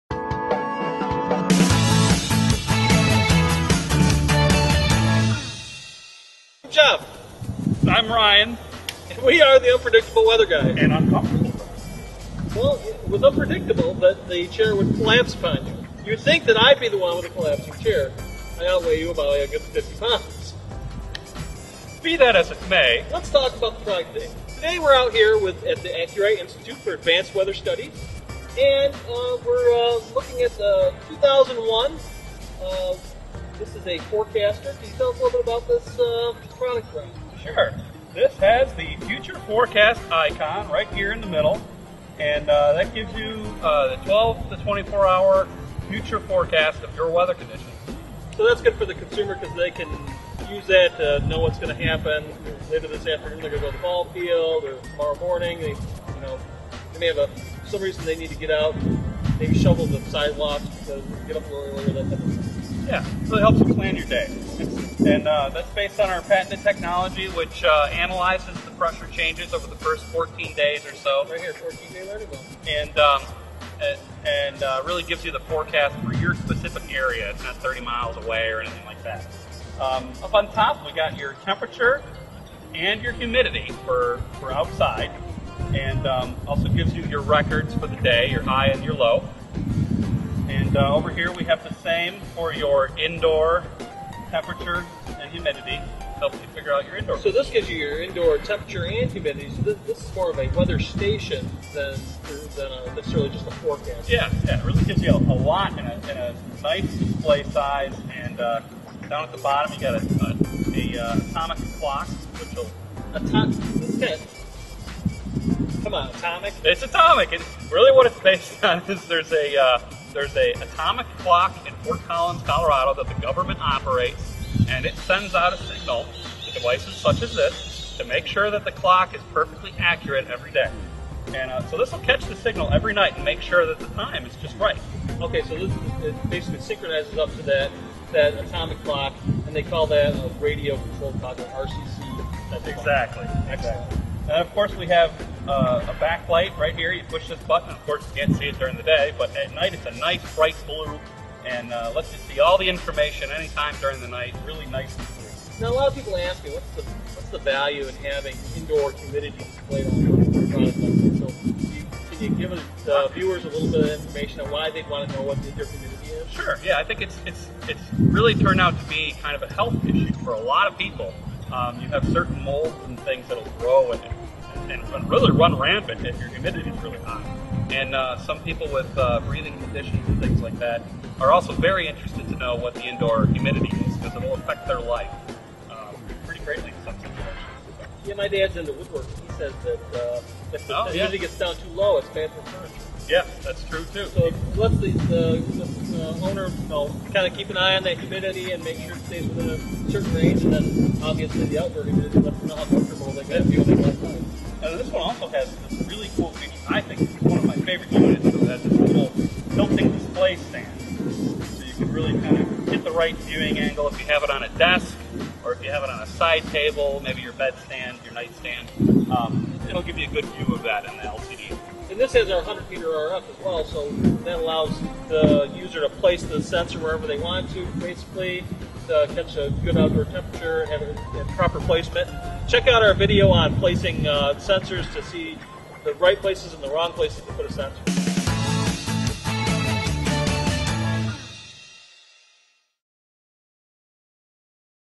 AcuRite Weather Forecaster 02001
The AcuRite Digital Weather Station generates a weather forecast while tracking temperature, humidity, time and date.

Weather the day with confidence with your own weather and weather prediction information -- pinpoint accuracy right from your own backyard. The AcuRite Digital Weather Station uses patented Self-Calibrating Technology to provide your personal forecast of 12 to 24 hour weather conditions. Self-Calibrating Forecasting is generated from weather data measured by a sensor in your yard - giving you the most accurate forecast available for your exact location.

The bold, easy to read LCD screen includes indoor / outdoor temperature and humidity, self-setting clock and calendar. Atomic clock and calendar sets itself to the correct time and date with split-second precision, and adjusts itself for Daylight Saving Time (DST). Display stands upright for tabletop use or is wall-mountable. The outdoor sensor features powerful wireless technology, low battery indicator light, and an integrated hanger for easy mounting. One-year limited warranty. It's more than accurate, it's AcuRite.